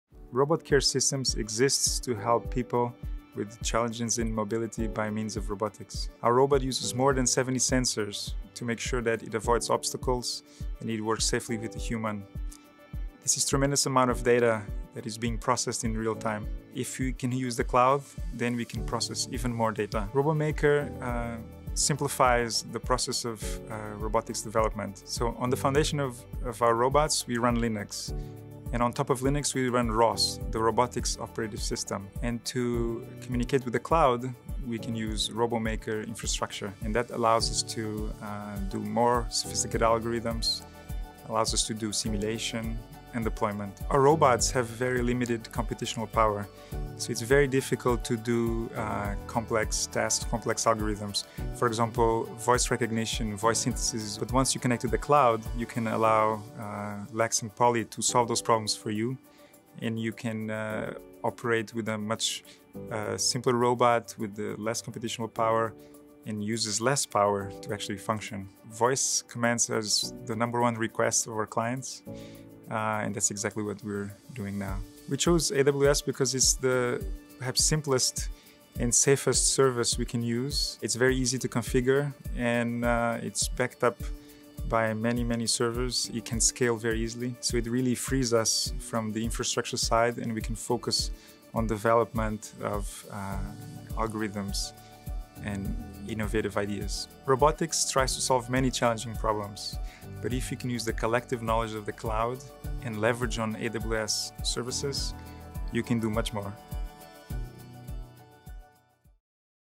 Robot Care Systems Case Study - Amazon Web Services
AWS RoboMaker exponentially increases the capabilities of Lea, an autonomous robot assistant for the elderly and disabled. Lea is interactive, keeps elderly safe and active, while it can talk to you, navigate around your house and keep you connected with family and doctors. We have used AWS RoboMaker cloud extensions for ROS to enhance Lea with video and telemetry data streaming, and voice interaction capabilities using services like Amazon Kinesis, Amazon Lex, and Amazon Polly. These cloud services and extensions provided by AWS RoboMaker have enabled us to rapidly develop new features, while breaking the limitations of small on-board computing power.